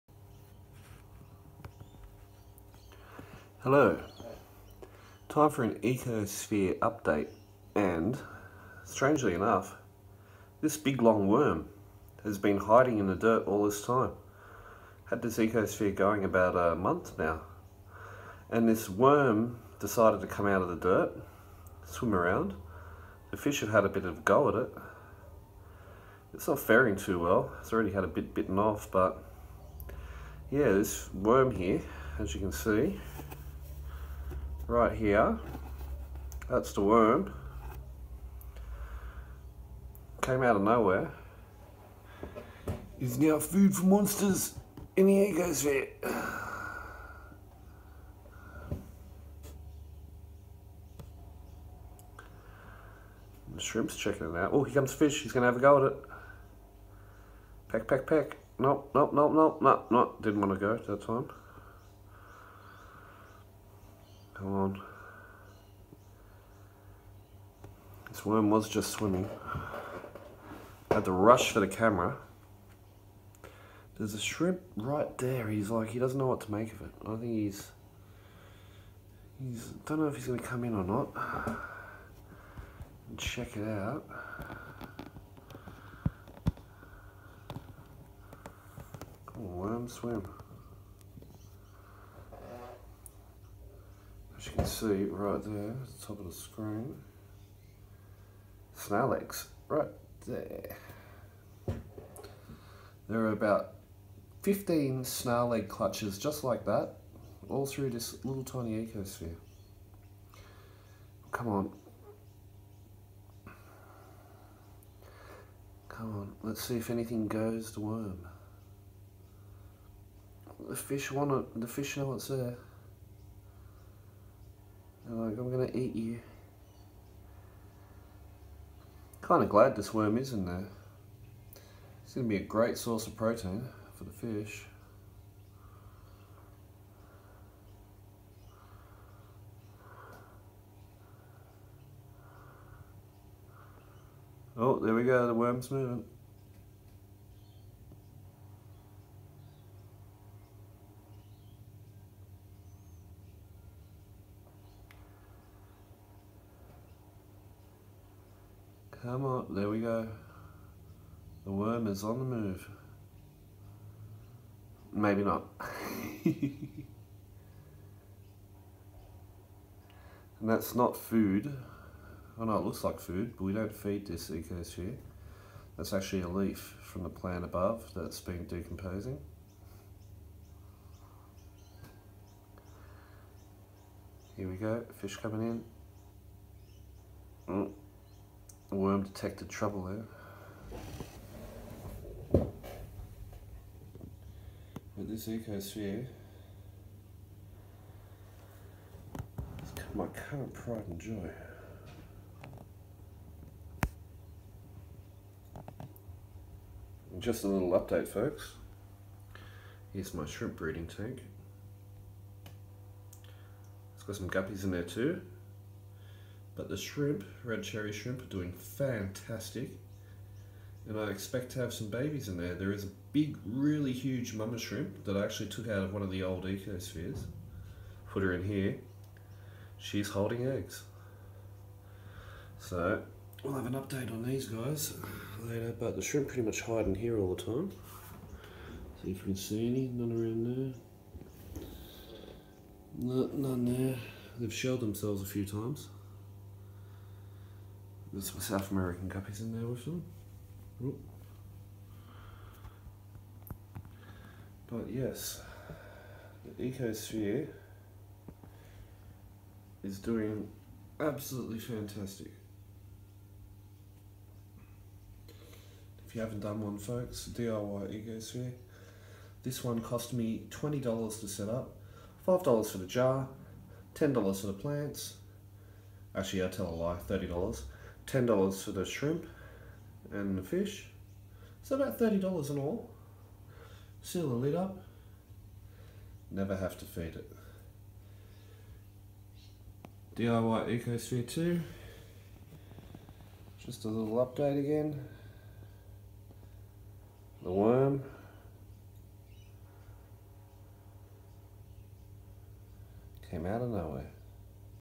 DIY Ecosphere Update. Big worm appeared out of nowhere
1 month old Ecosphere. Huge worm appeared this morning swimming in the Ecosphere